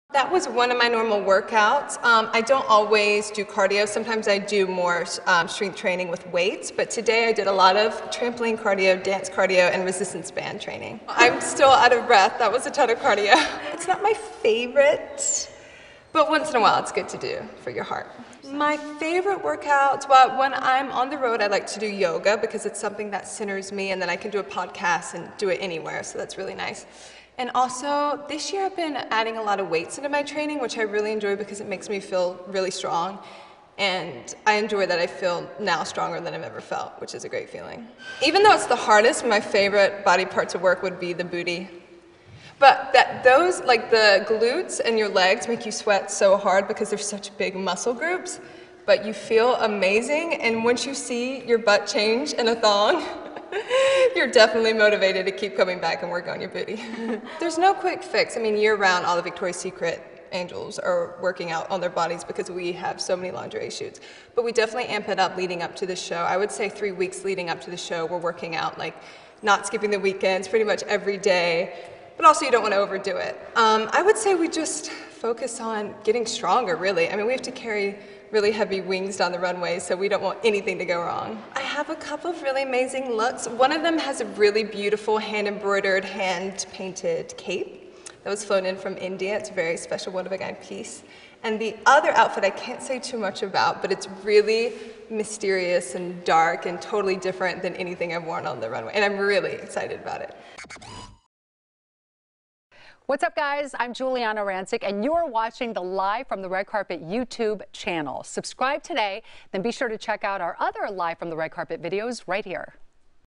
How to Train Like a Victoria's Secret Angel | E! Red Carpet & Award Shows
Victoria's Secret model Martha Hunt shares her intense cardio & strength training workouts. Plus, find out her favorite exercise to do when traveling.